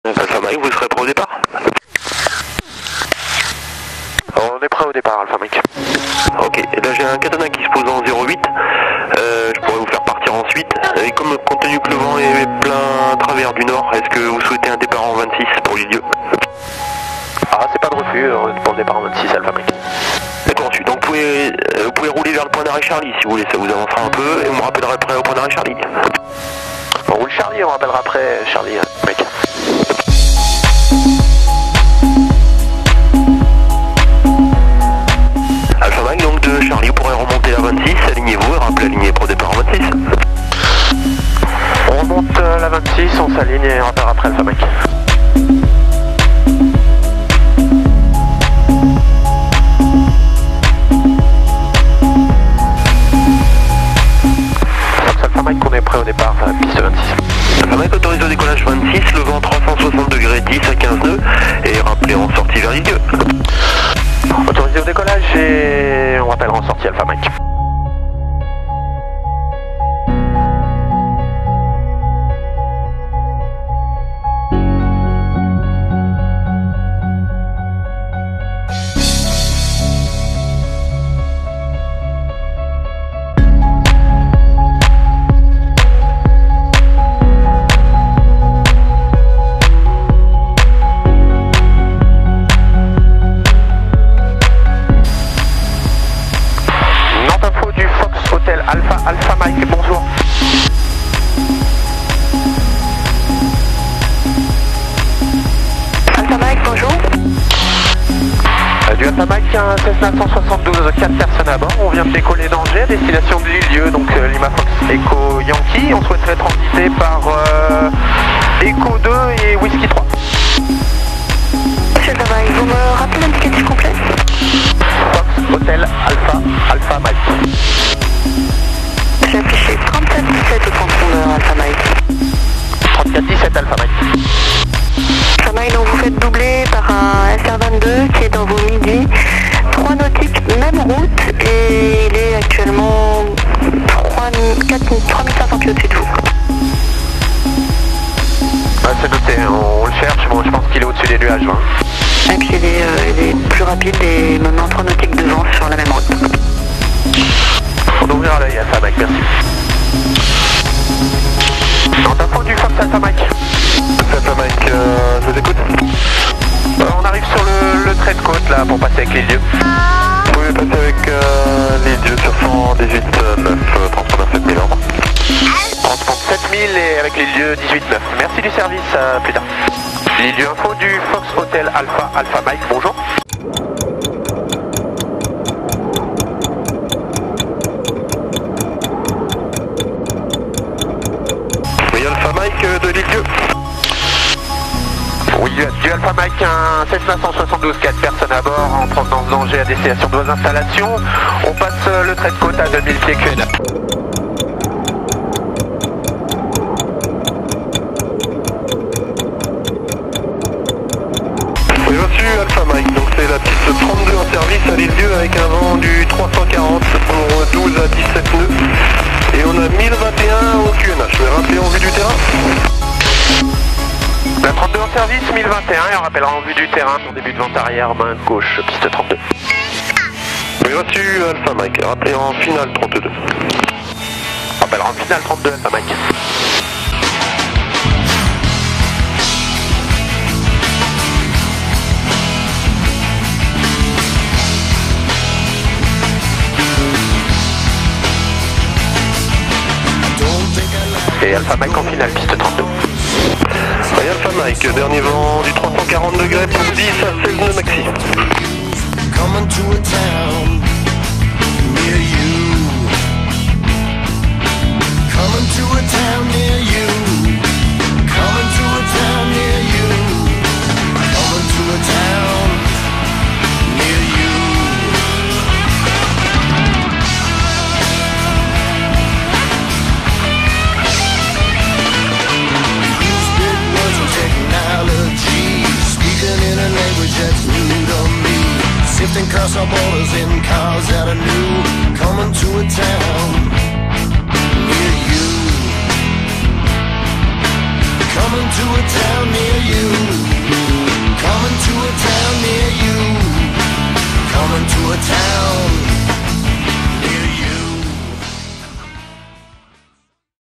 Un petit tour à l'Ile d'Yeu?
récit d'un vol entre Angers (LFJR) et l'Ile d'Yeu (LFEY)...